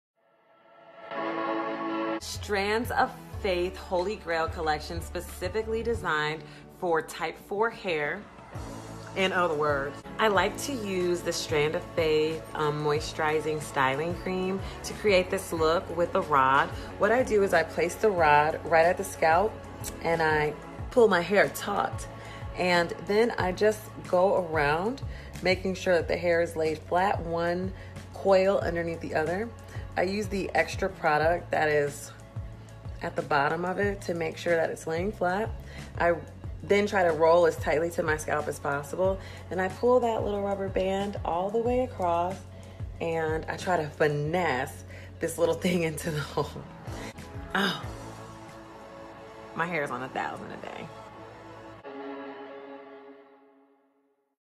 Moisturized Perm Rod Set
Suffering from dry and dehydrated hair? Watch as @ceci_honeycomb_brown show you how to revive your tresses using the Strands of Faith Holy Grail Moisturizing Cream!